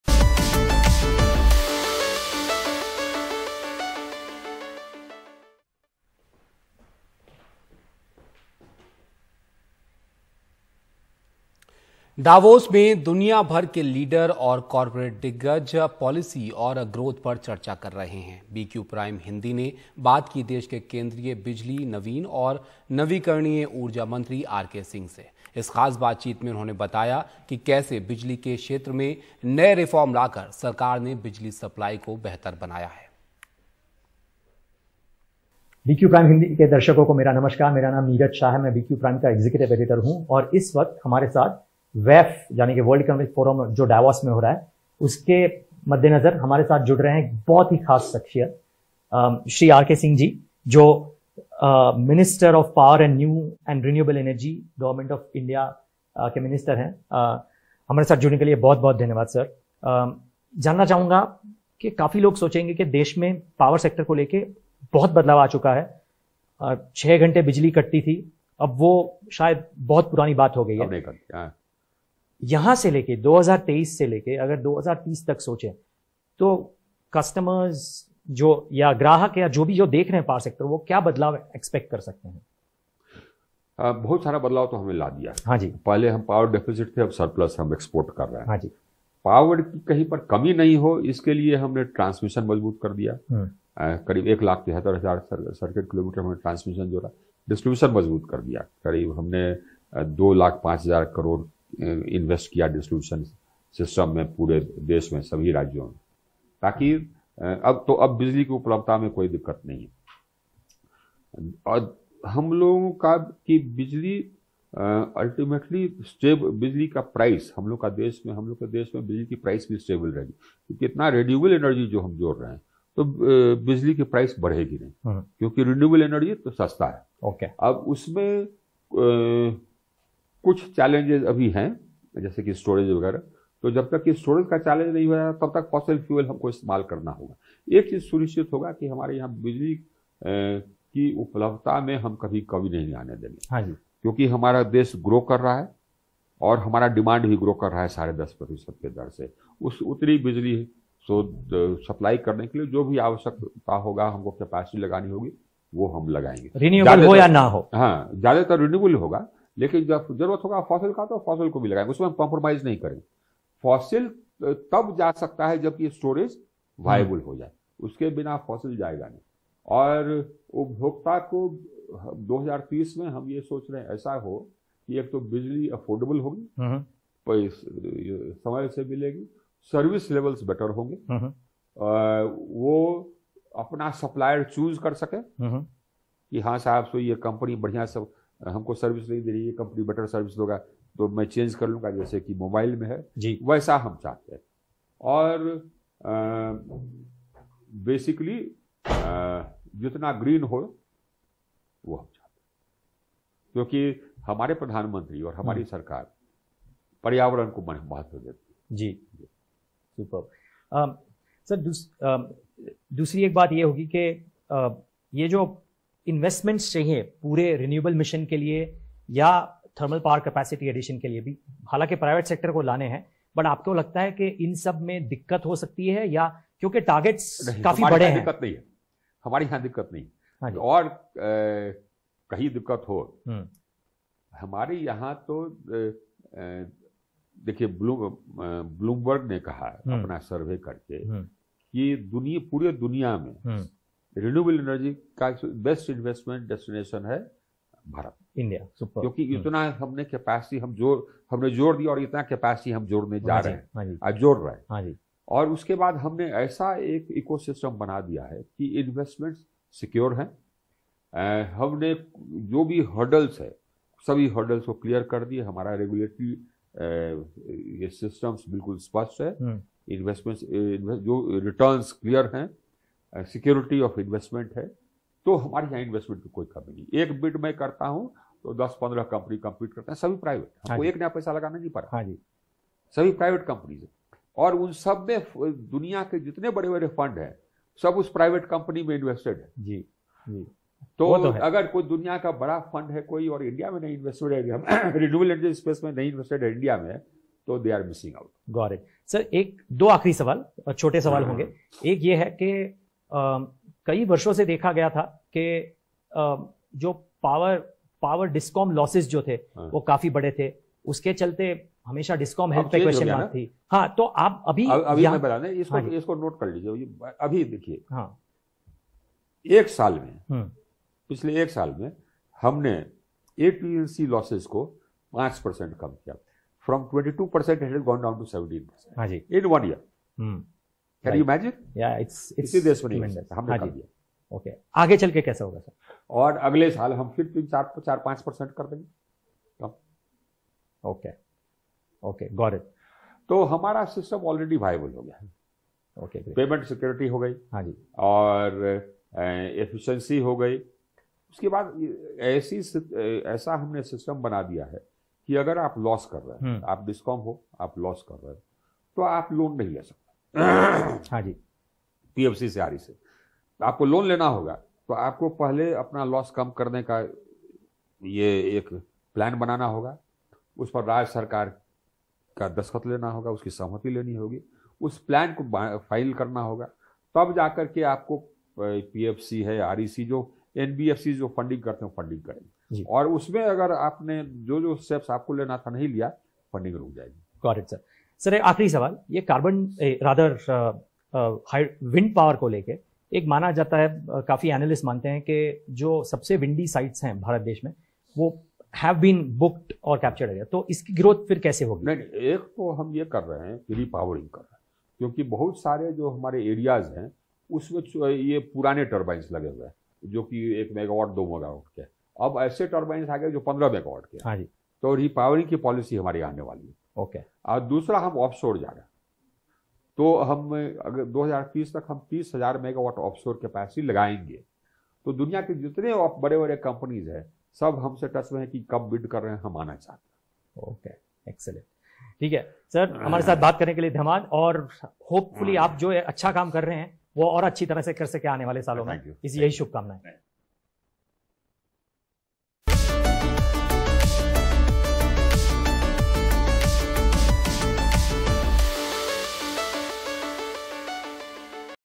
ऊर्जा मंत्री RK Singh ने कहा - "सुधारों से बिजली Sector में आई तेजी"
दावोस (Davos) में दुनिया भर के लीडर और कॉरपोरेट दिग्गज पॉलिसी और ग्रोथ पर चर्चा कर रहे हैं. इसी क्रम में BQ हिंदी ने बात की देश के केंद्रीय बिजली, नवीन और नवीकरणीय ऊर्जा मंत्री आरके सिंह (RK Singh) से. इस खास बातचीत में उन्होंने बताया कि कैसे बिजली के क्षेत्र में नए रिफॉर्म लाकर सरकार ने बिजली सप्लाई को बेहतर बनाया है.